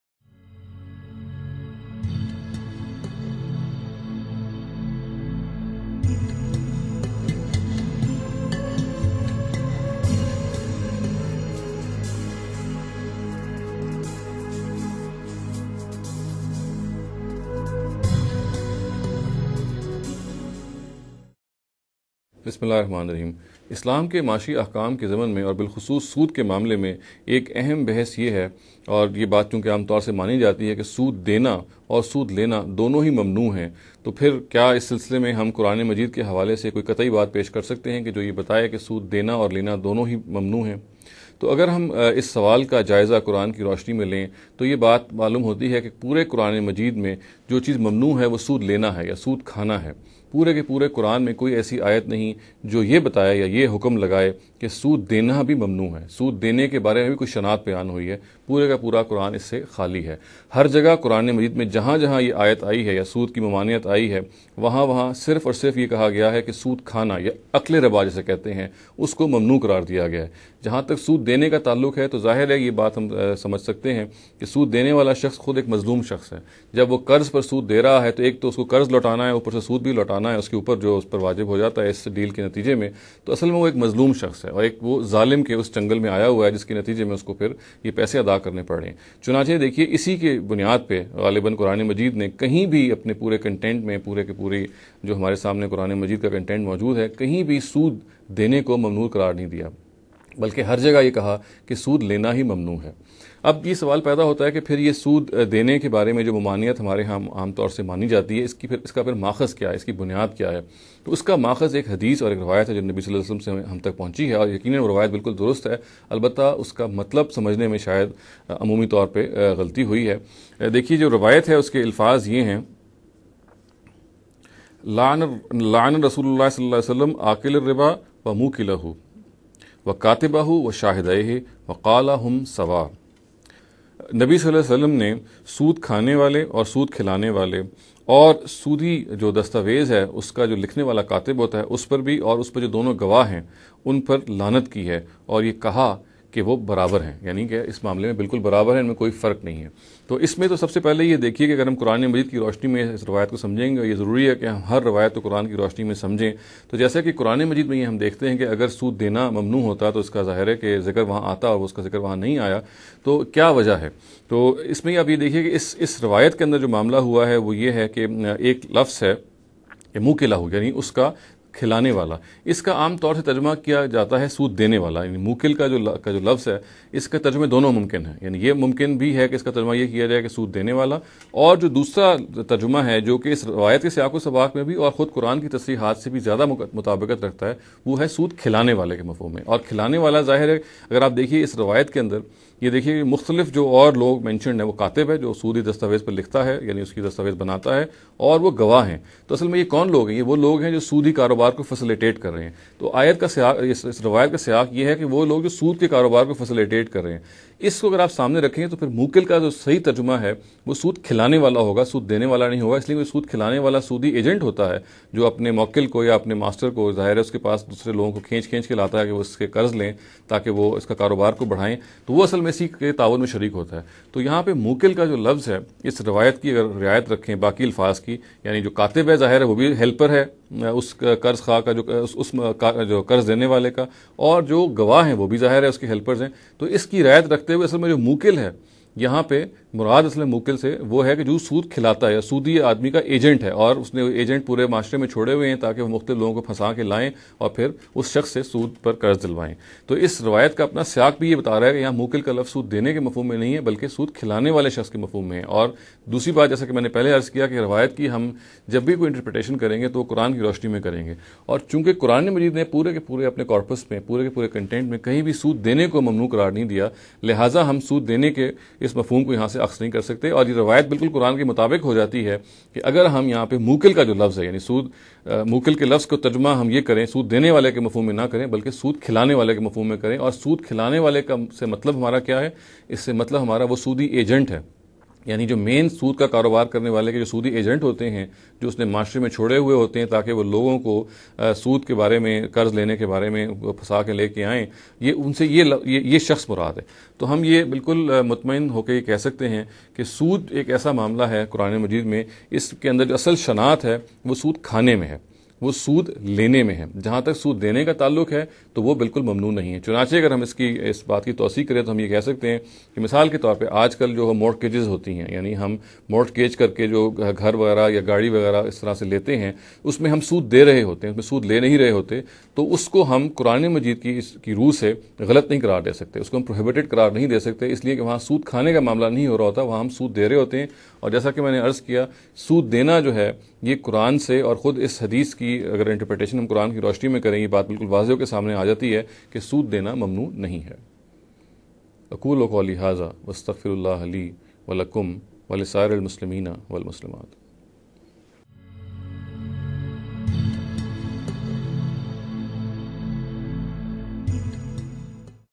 Kiya Sood Intrest Daina bhi Mamnoo hai Chand Galat Fehmiyan
Shahzad Saleem (a student of Javed Ahmed Ghamidi) is clarifying various misconceptions about Islam.